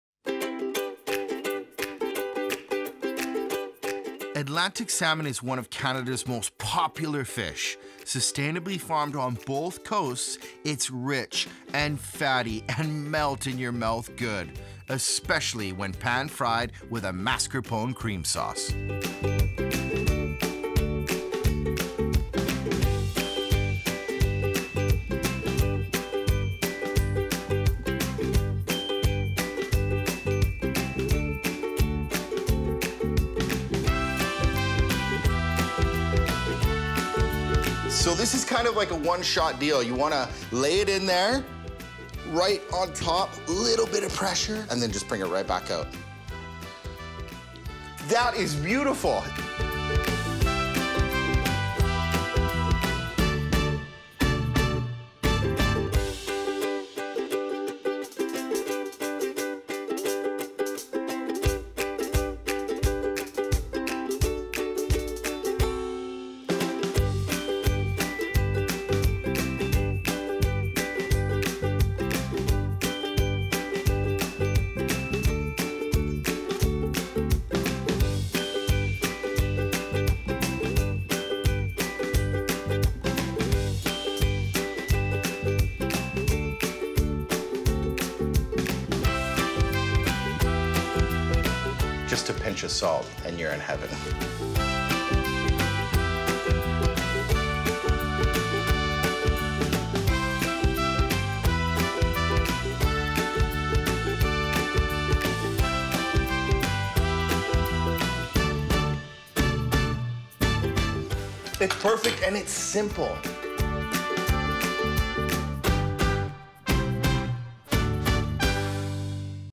Easy Pan Seared Atlantic Salmon with Cream Sauce
- A simple pan seared salmon recipe with a creamy mascarpone sauce and garlic herb coating